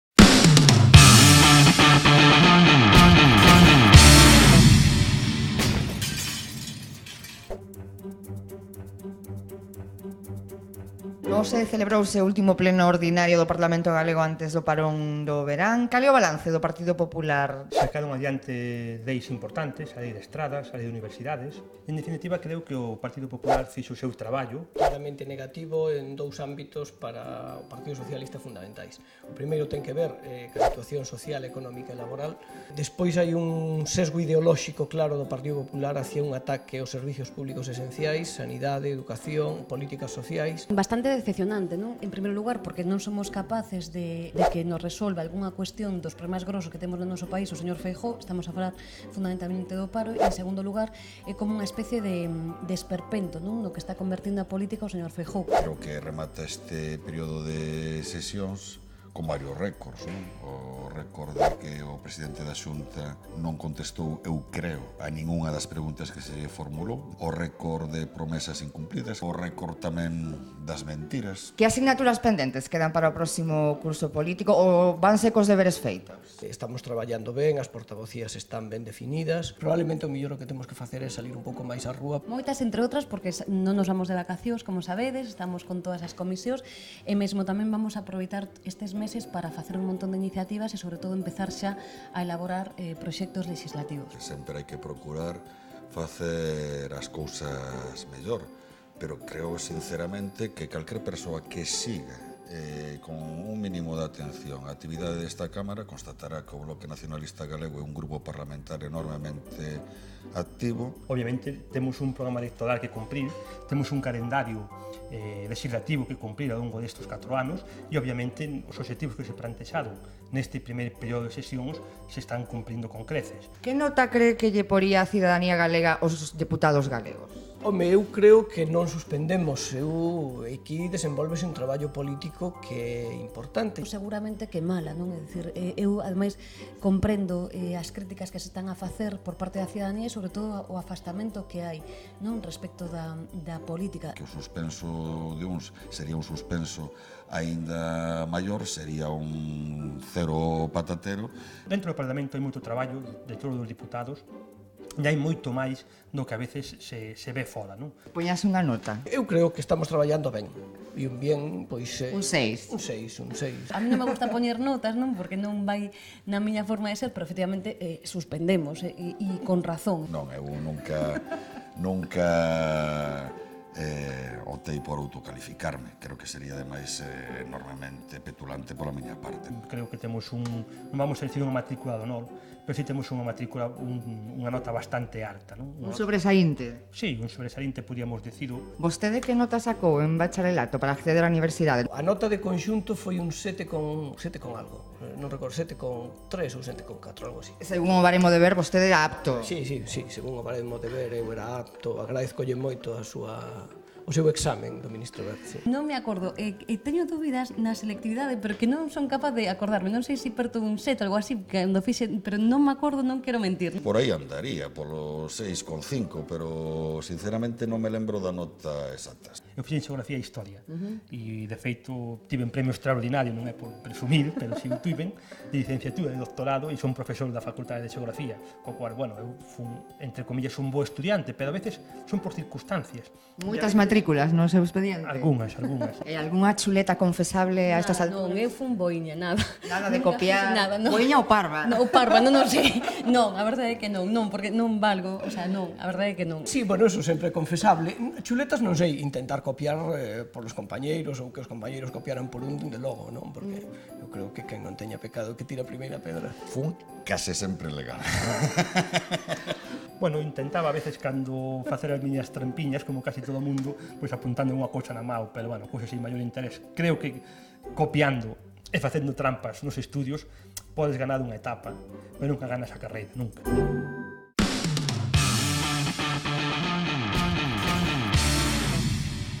As notas dos nosos deputados
das polémicas políticas da semana foi a intención do ministro Wert de instaurar o 6,5 como nota de corte para acceder ás becas universitarias. Un asunto que esta semana trasladamos ao Parlamento de Galicia. Por uns instantes as señorías cambiaron o escano polo pupitre para falarnos das súas experiencias académicas.